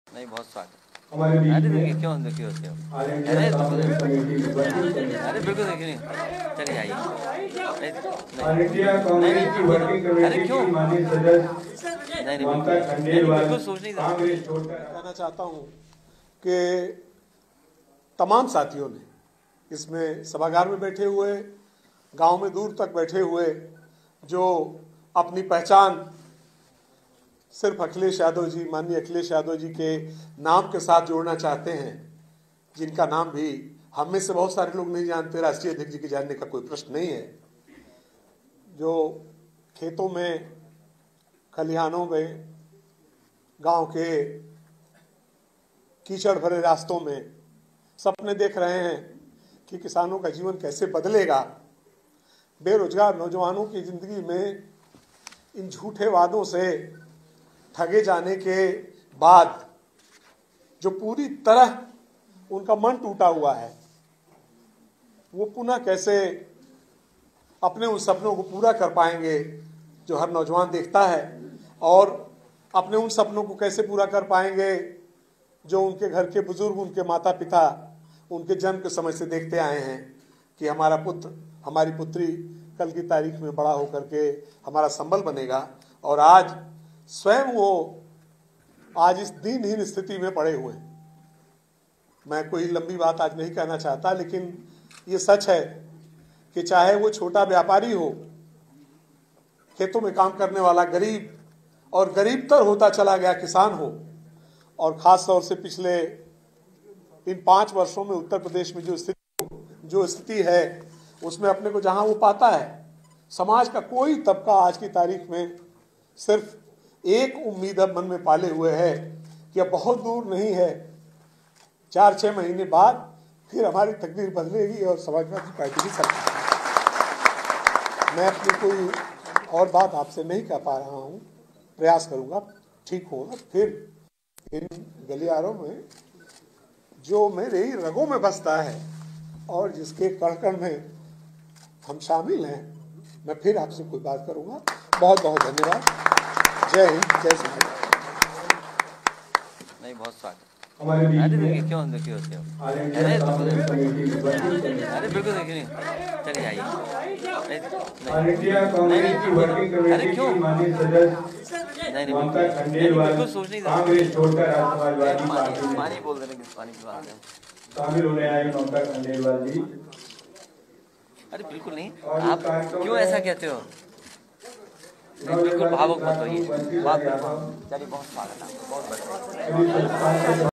अखिलेश यादव के सामने भाषण देते देते रोने लगे अम्बिका चौधरी || Samajwadi Akhilesh
Former Samajwadi Party minister Ambika Chaudhary joined the Samajwadi Party as well as gave a speech in front of Akhilesh Yadav.